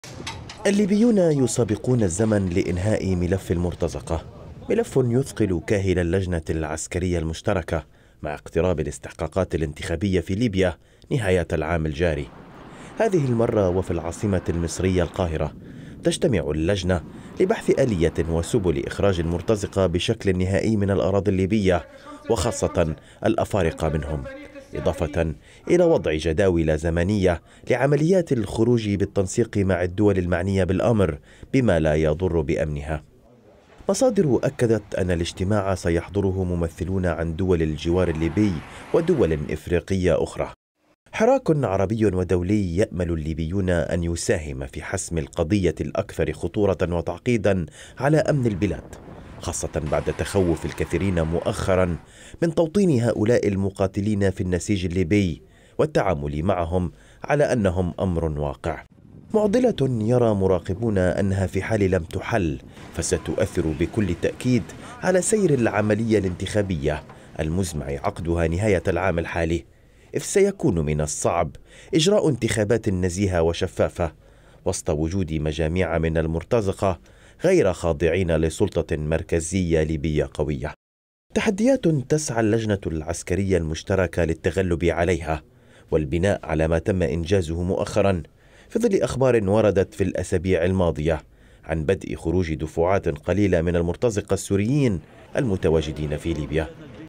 ليبيا.. انطلاق اجتماع لجنة 5+5 بمصر لبحث إخراج المرتزقة
انطلقت في العاصمة المصرية القاهرة، اجتماعات اللجنة العسكرية الليبية المشتركة خمسة زائد خمسة، لبحث إخراج المرتزقة والقوات الأجنبية والمقاتلين الأجانب من البلاد، بما يساهم فى دعم الحلول السياسية.
ويتم الاجتماع الثانى للجنة العسكرية الليبية المشتركة فى مصر، بمشاركة المبعوث الأممي لدى ليبيا يان كوبيش، وممثلي عن دول جوار ليبيا.